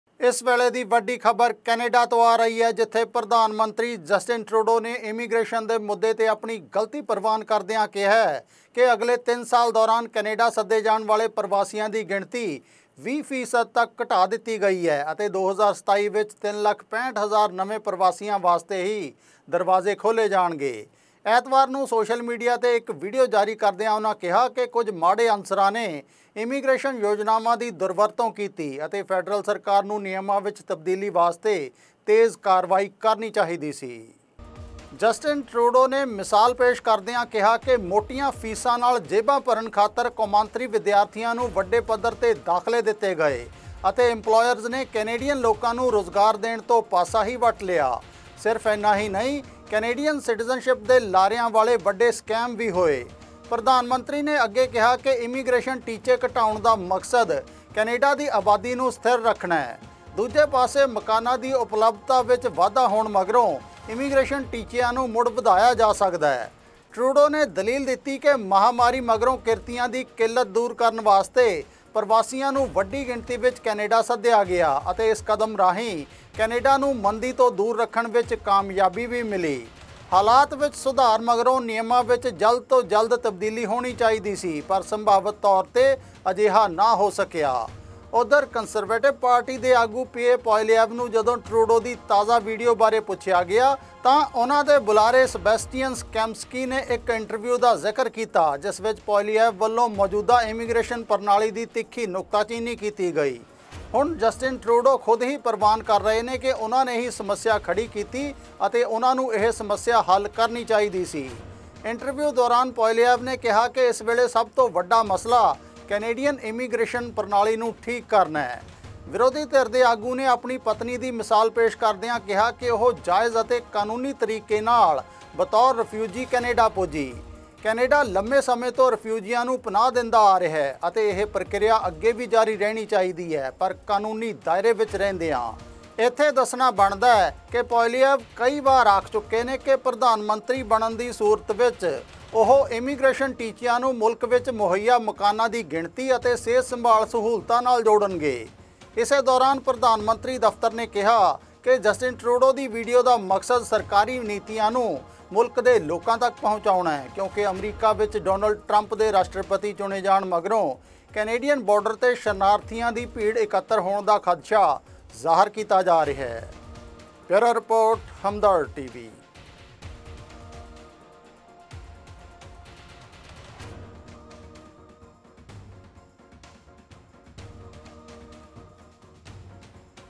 Canada ਆਉਣ ਵਾਲਿਆਂ ਦੀ ਗਿਣਤੀ ਵਿਚ 20 ਫੀ ਸਦੀ ਕਟੌਤੀ ਦਾ ਐਲਾਨ, Trudeau ਨੇ ਇੰਮੀਗ੍ਰੇਸ਼ਨ ਦੇ ਮੁੱਦੇ ’ਤੇ ਗਲਤੀ ਮੰਨੀ
ਜਸਟਿਨ ਟਰੂਡੋ ਨੇ ਇੰਮੀਗ੍ਰੇਸ਼ਨ ਦੇ ਮੁੱਦੇ ’ਤੇ ਗਲਤੀ ਮੰਨੀ
ਕੈਨੇਡਾ ਆਉਣ ਵਾਲਿਆਂ ਦੀ ਗਿਣਤੀ ਵਿਚ 20 ਫੀ ਸਦੀ ਕਟੌਤੀ ਦਾ ਐਲਾਨ
2027 ਵਿਚ 3 ਲੱਖ 65 ਹਜ਼ਾਰ ਪ੍ਰਵਾਸੀਆਂ ਨੂੰ ਹੀ ਸੱਦਿਆ ਜਾਵੇਗਾ